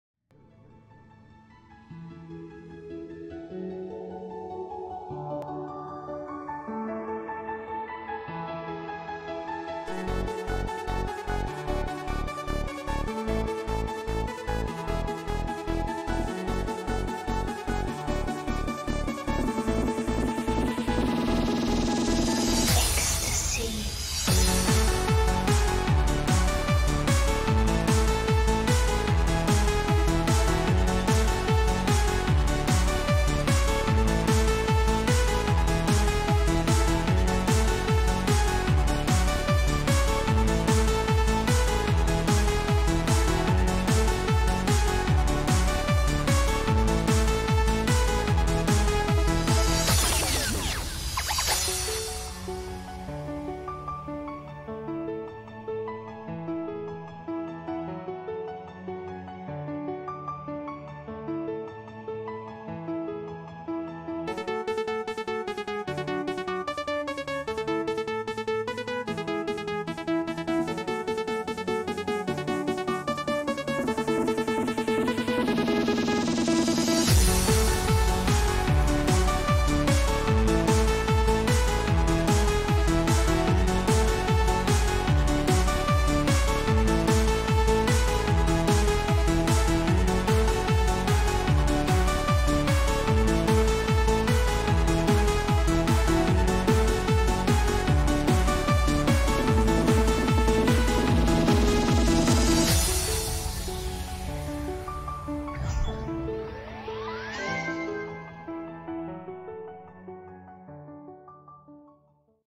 Glacial Fragility [Full layout]
Zephlin layout 2

I wanted to do something kinda basic, galatic fragility inspired obviously but with some modern mechanics (robot/spider) This only took me a week and I never plan on decorating this atm.

I have more layouts and collab parts soon (also a new solo level hoping to finish it this summer!